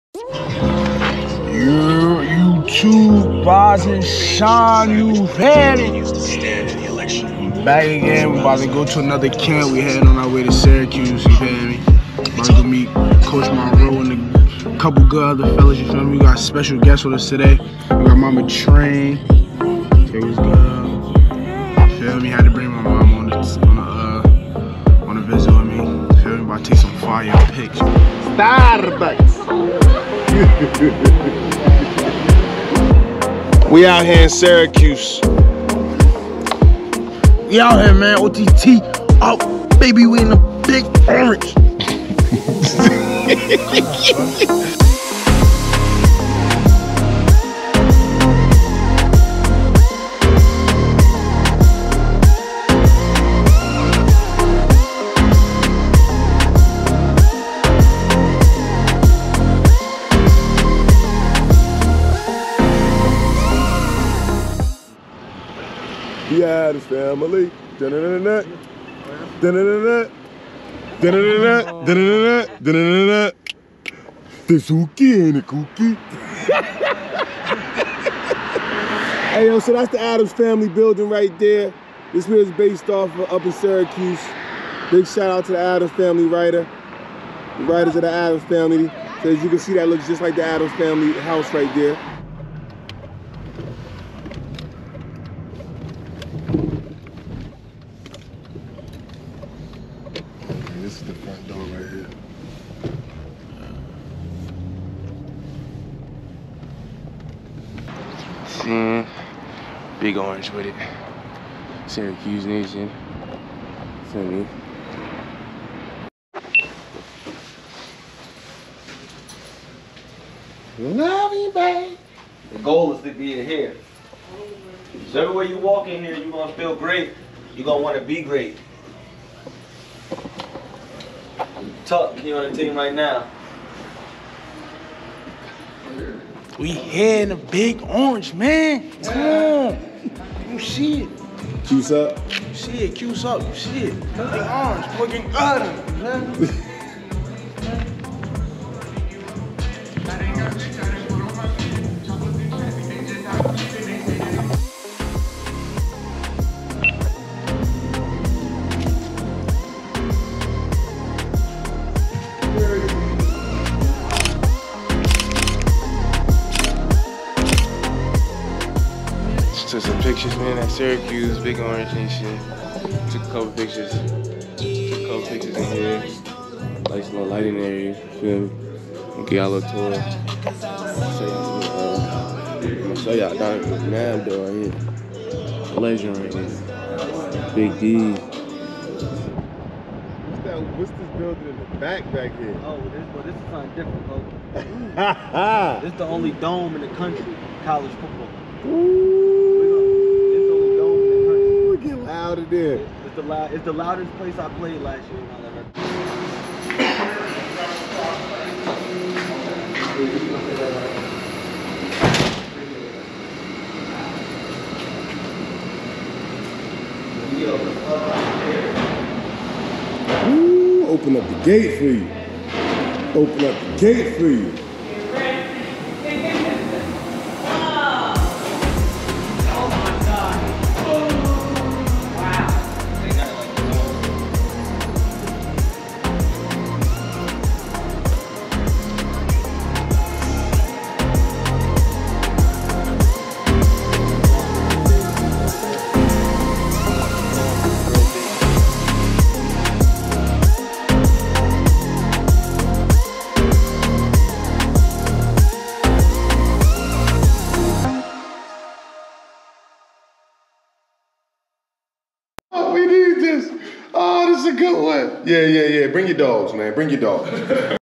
VLOG | OTT BOYZ TOUR SYRACUSE UNIVERSITY
Take another ride with the OTT Boyz as they visit the Big Orange! We take a tour of the campus, facilities, and stadium and give you a behind the scenes look at Syracuse Football.